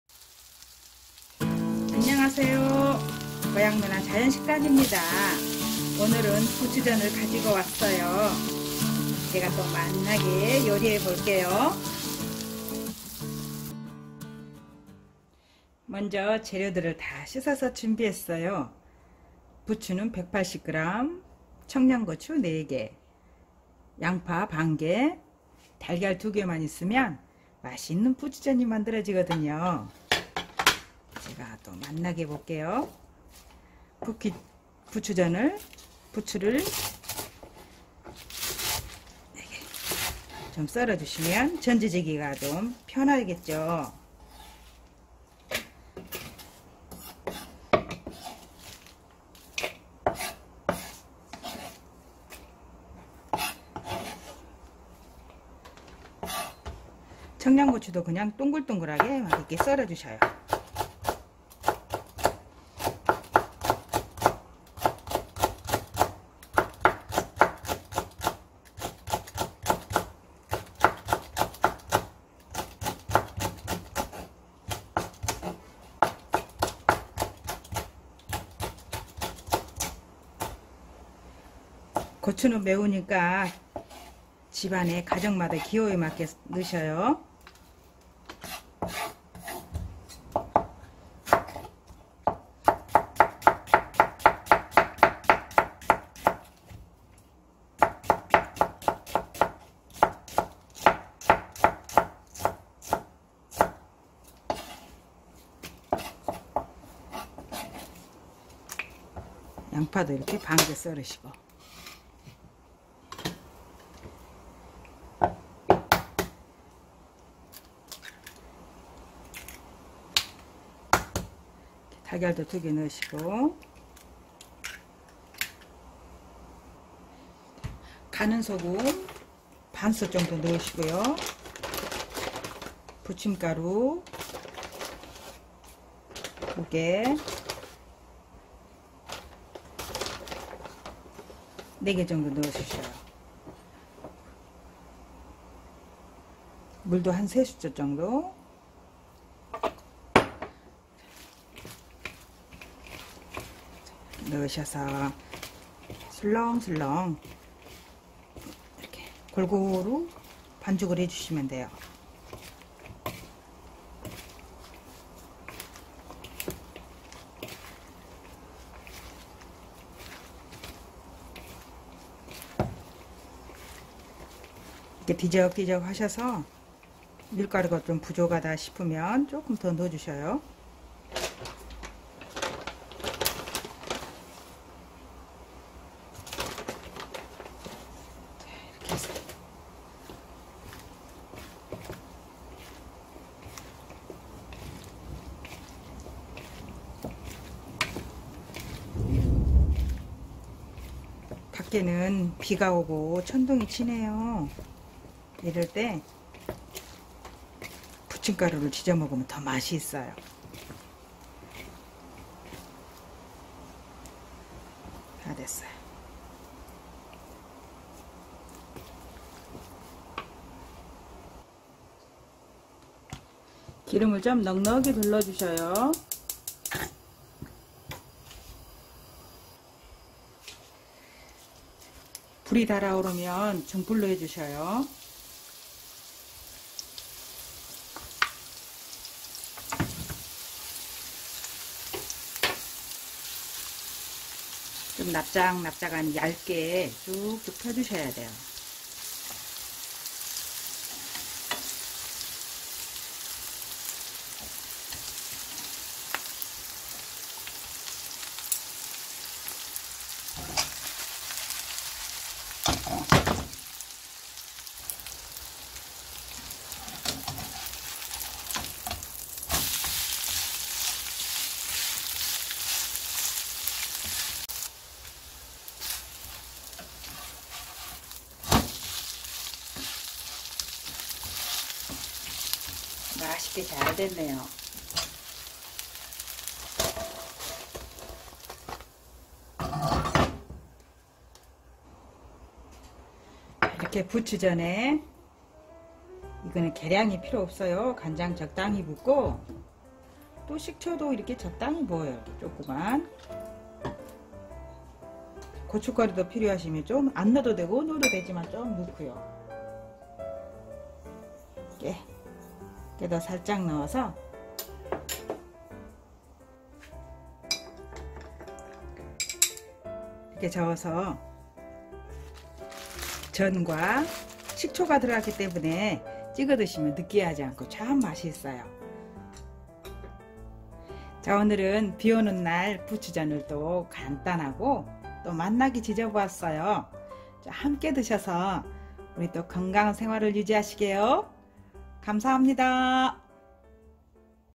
비오는날 쉽게 [부추전]부추전 맛있게 만드는 방법, 부추전 맛있게 부치는법
*레시피*
부추180g
청량고추 4개
양파 반개
닭걀 2개
가는소금 반수저
부침가루 6수저
물 3수저
식용류

양념
긴간장 적당 3수저
식초 적당 1수저
고추가루조금 수저끝
깨